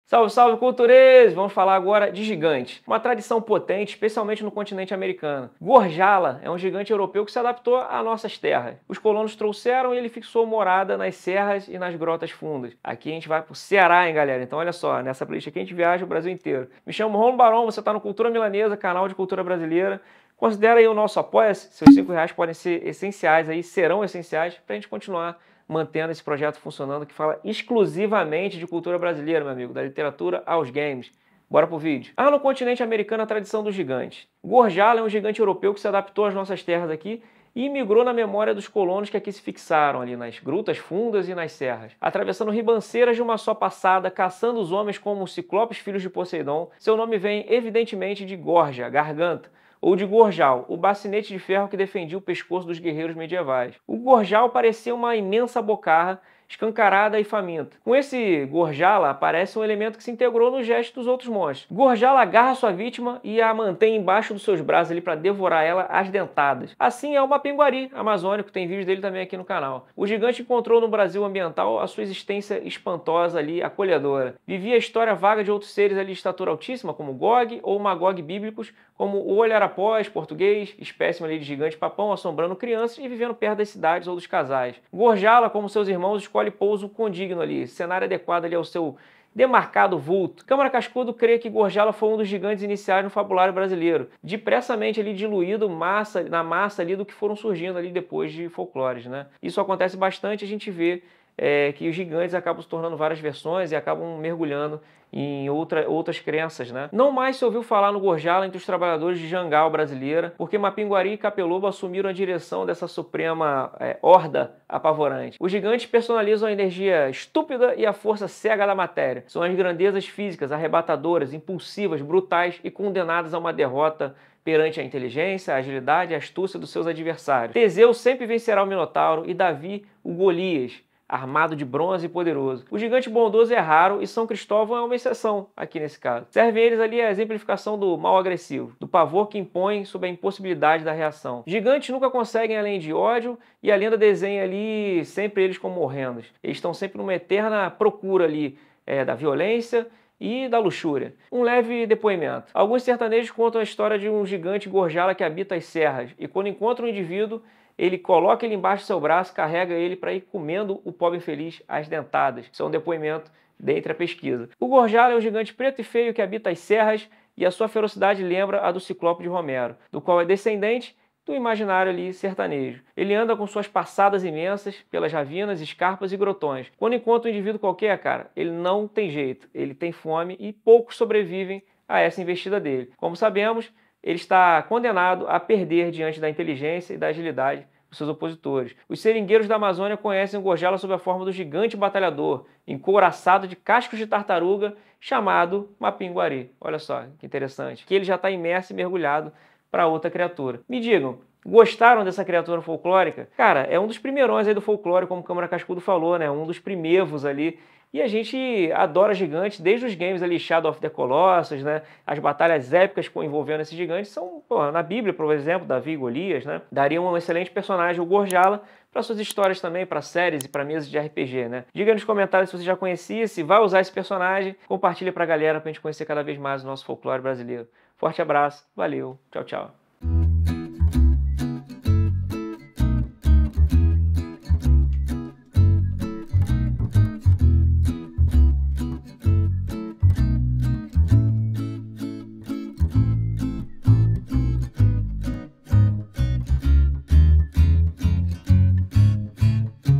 GORJALA: FOLCLORE BRASILEIRO. Cultura à Milanesa
Gorjala é um dos personagens folclóricos ancestrais, daqueles primeiros que depois foram diluídos nos CAPELOBOS e MAPINGARIS.